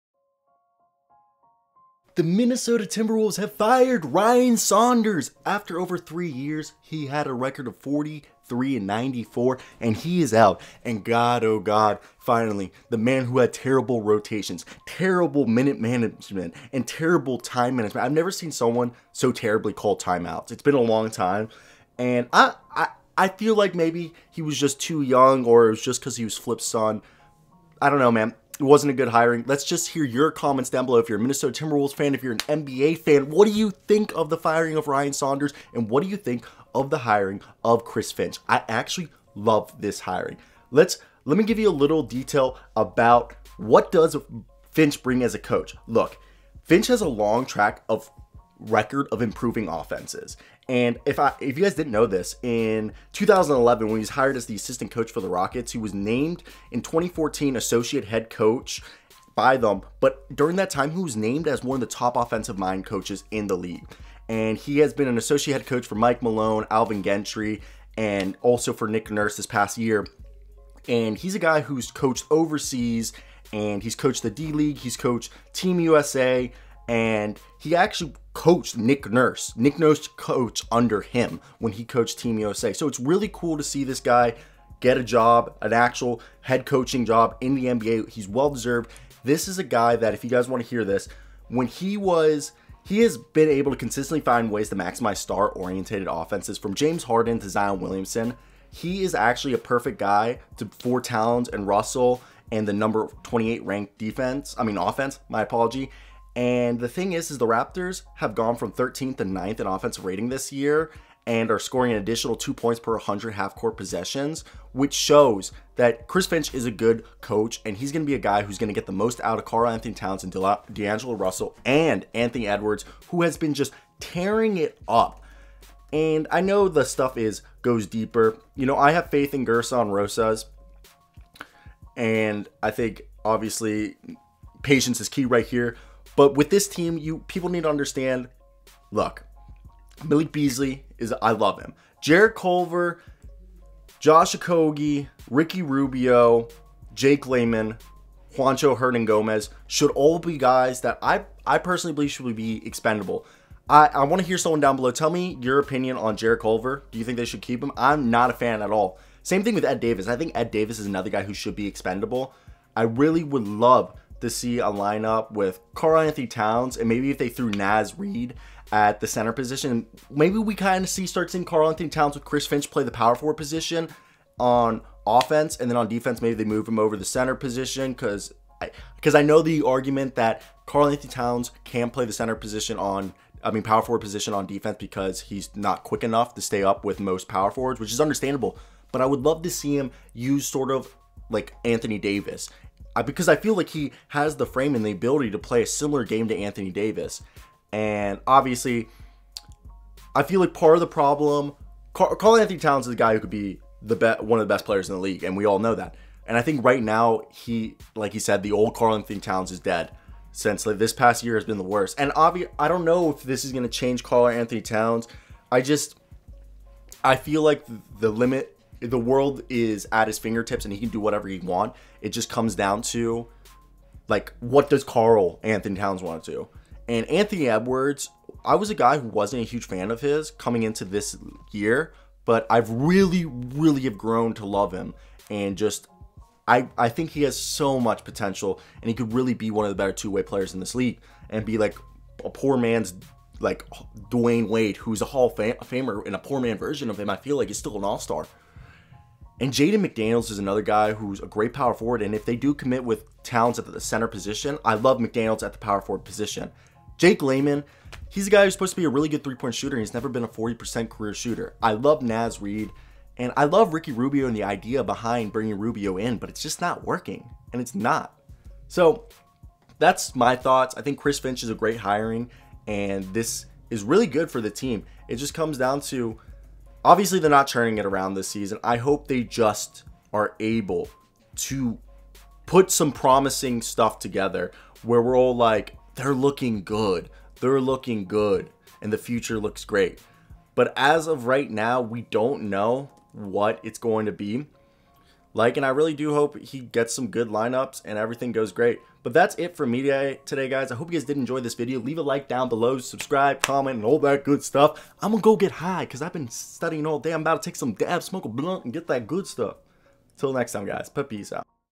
Timberwolves Fire Ryan Saunders & Hire Chris Finch So What's Next?
Timberwolves Fire Ryan Saunders & Hire Chris Finch so what is next? The Minnesota Timberwolves fired head coach Ryan Saunders late Sunday night after over three seasons compiling a 43-95 record and will hire Toronto Raptors assistant Chris Finch on a multi-year contract to replace Saunders, according to The Athletic.